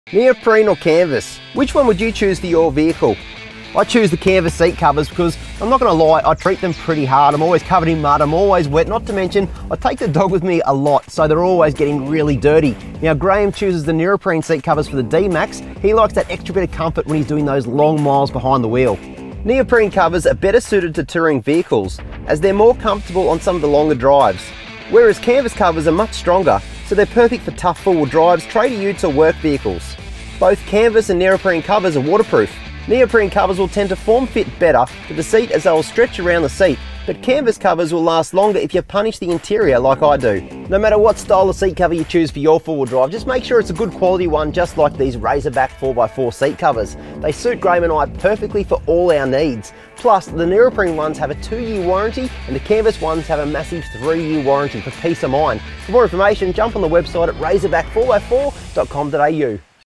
Neoprene or canvas seat covers?
Wondering whether neoprene or canvas seat covers are the go? Shauno takes you through best use cases for both!

Grab yours on our website at razorback4x4.com.au!